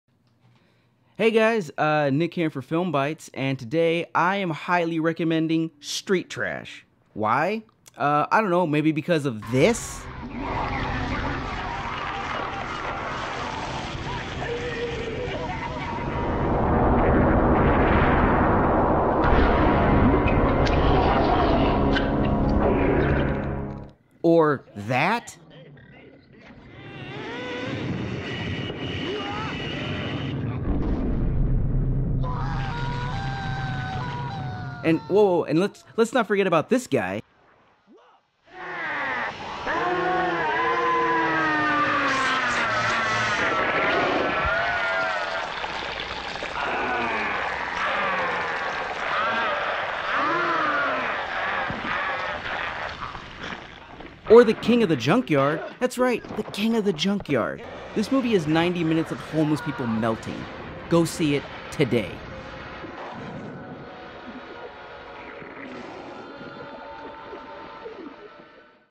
Street Trash (1987) | Horror Bites
Nick shows you why you should see the most gruesome movie we've watched this Marathon, Street Trash. Basically it's just a bunch of homeless people melting.

Day 7 of our 31 days of October horror movie review marathon.